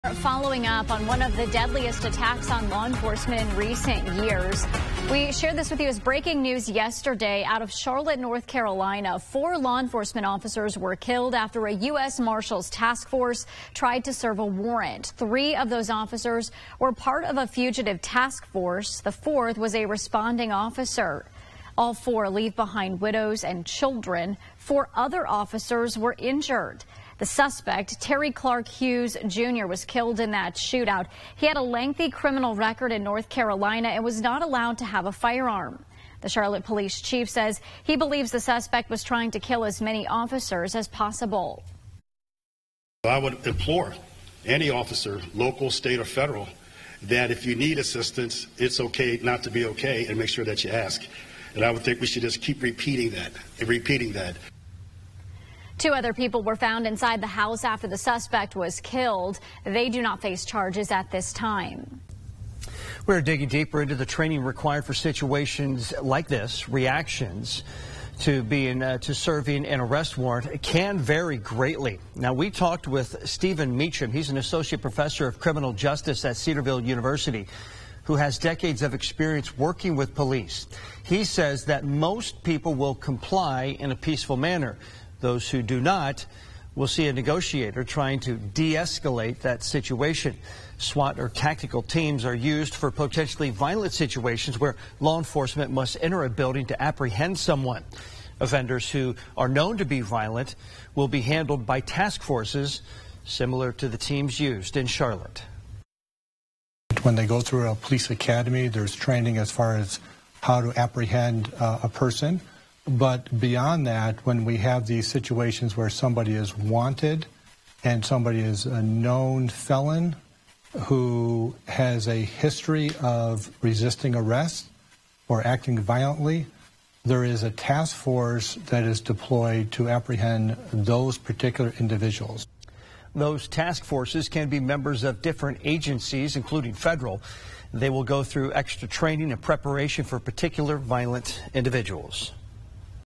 Charlotte Police Officers Killed from Task Force
Four Charlotte, North Carolina police officers were killed on duty during a task force attempt to serve a warrant. With these tragic deaths, there have now been 55 police officers killed in 2024--24 in April.